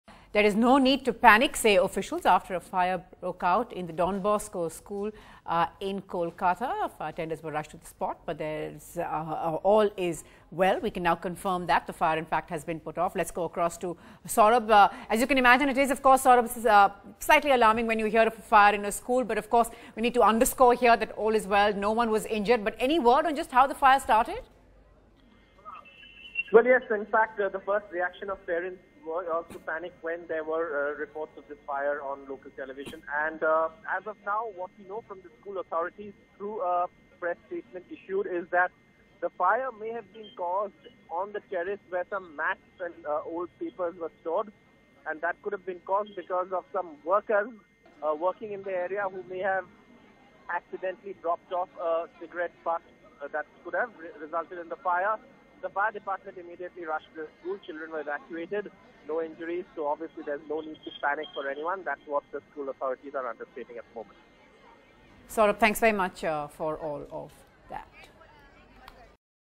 Fire Breaks Out At Lunchtime In Kolkata's Don Bosco School
A fire broke out at the well-known Don Bosco school in Kolkata this afternoon, during lunchtime. The fire reportedly started on the roof of the building, which is in the Park Circus area of central Kolkata. Close to 3000 students were rushed out of the building. Four fire engines were deployed but school authorities said the fire was brought under control in less than an hour before the engines arrived.